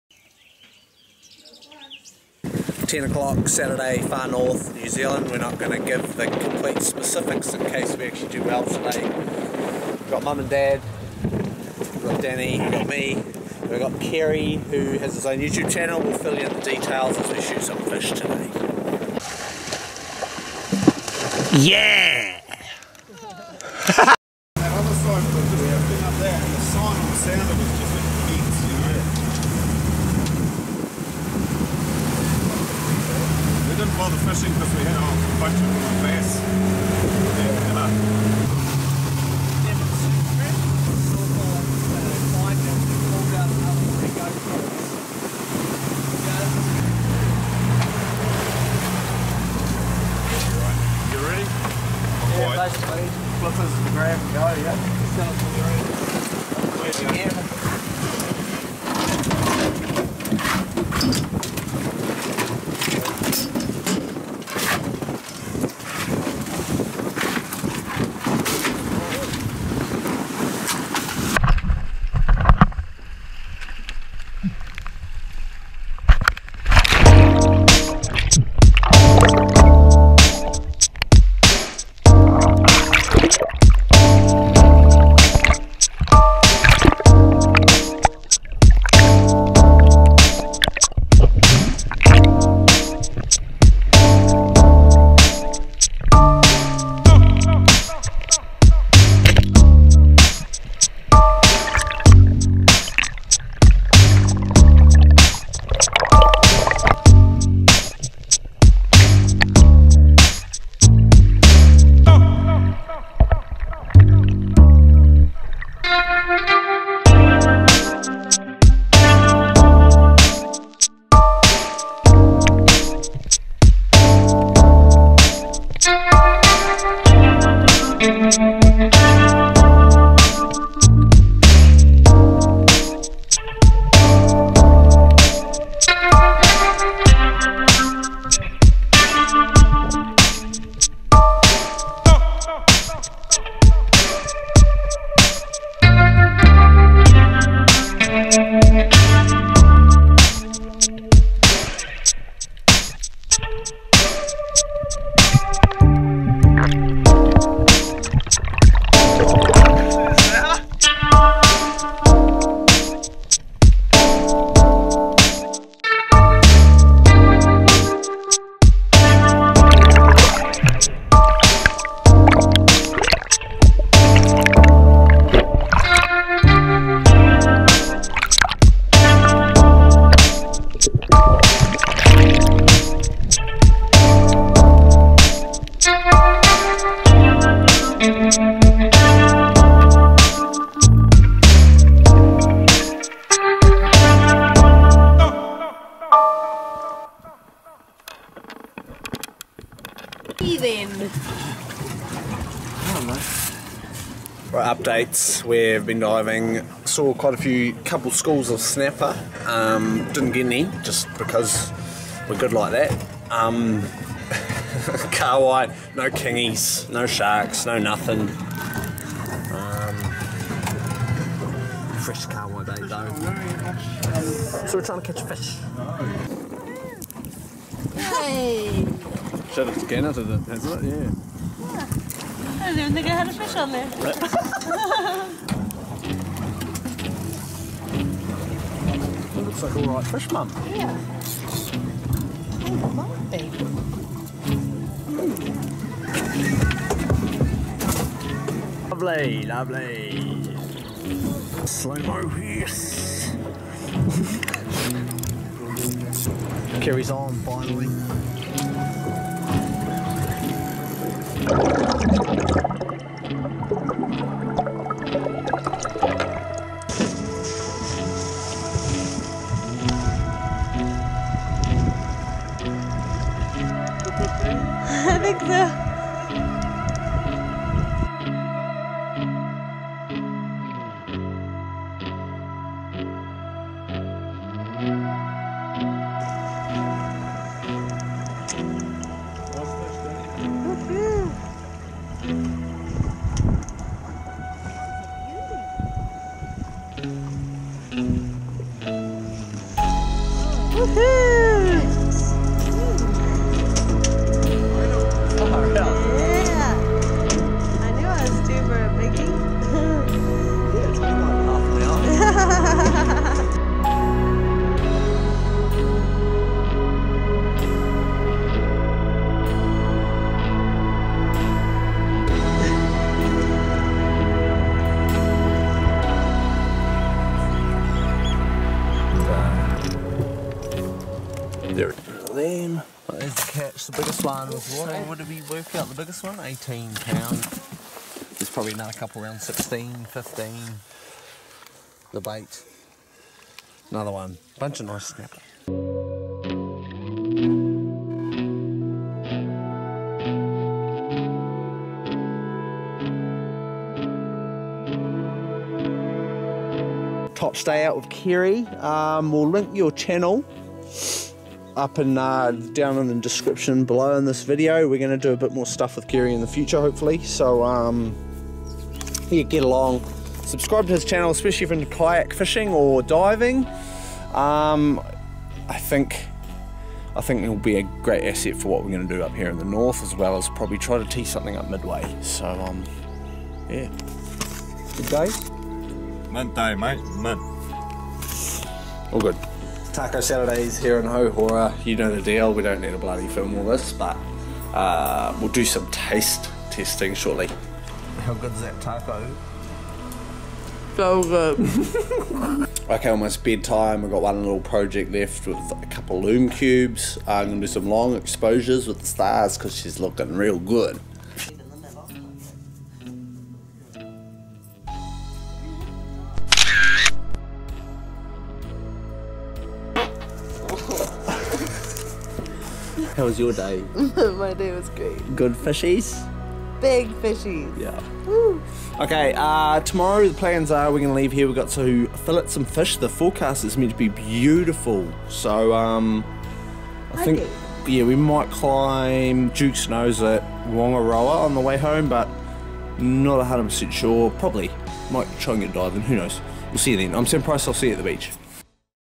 When Your Girlfriend Catches A Couple Of Snapper
Solid day in/out on the water. Found a few Far North pannies.

If you're into fishing, diving and more specifically Kayak fishing, then check out Kerry's channel